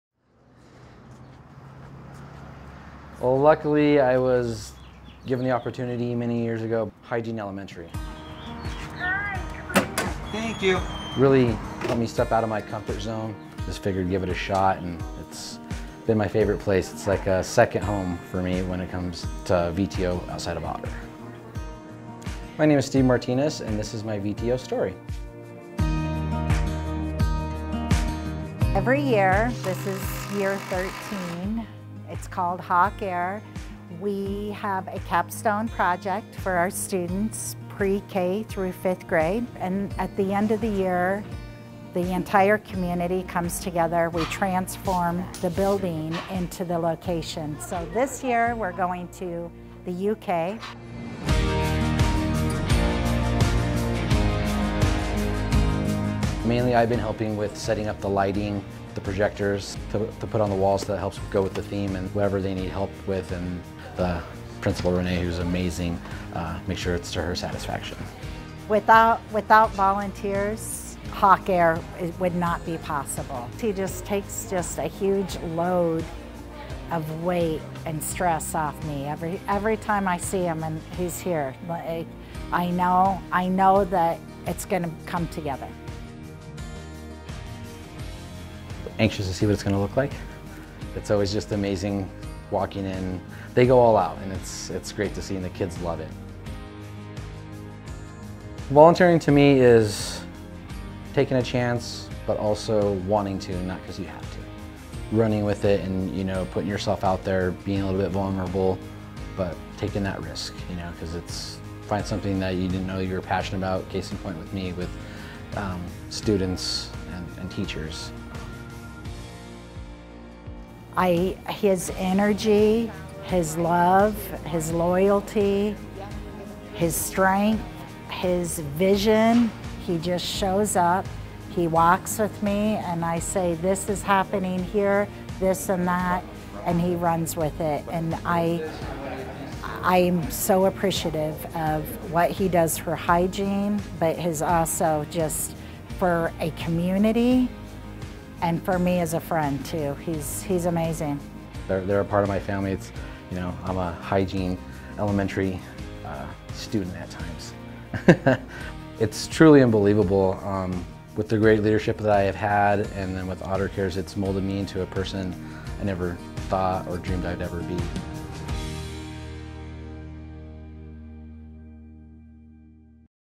VTO Spotlight - Steve Martinez
Otter Products employee Steve Martinez stumbled across a volunteer opportunity over a decade ago that has become his passion. Every year, Steve volunteers at Hygiene Elementary to help transform their school into an immersive educational experience for students and the community. From helping to decorate the various rooms in the school to setting up projectors to bring images of far away places to students, Steve uses all 24 of his volunteer hours at Hygiene to bring these experiences to life! Check out his story!